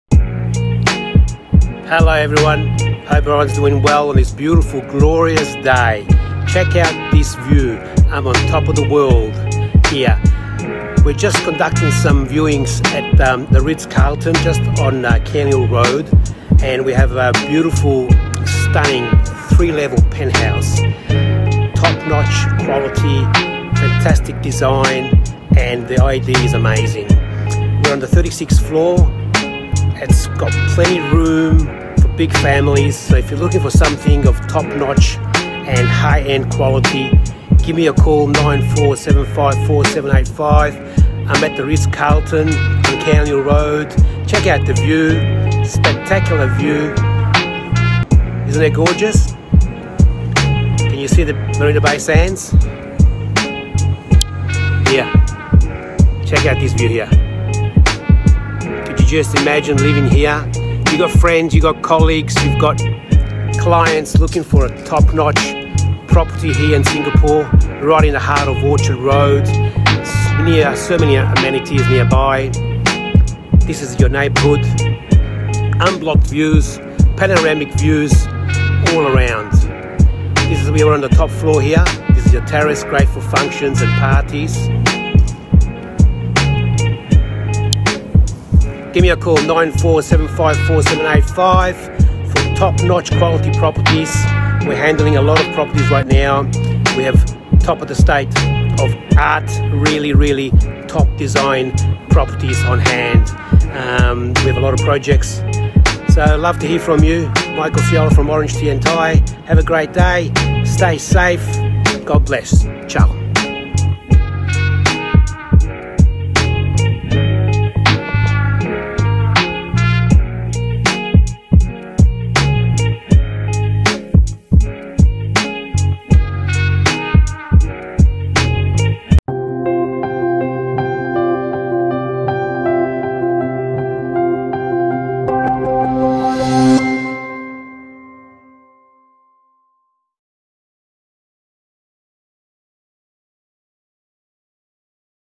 The Ritz-Carlton Residences For Sale
Additional 2 penthouses of 6,501sqft each (private pool with awesome Marina Bay Sands view)

Also available for sale
3 bedroom - 2831 sqft
4 bedroom - 3057 sqft

Ritz-Carlton Residences stands on a league of its own with the quality of comfort and luxury it offers its residents.

This freehold property is located at 65 Cairnhill Road Singapore in District 9.

Home seekers who prioritize comfort and luxury will find this project very appealing as it incorporates all that is needed to call a luxurious apartment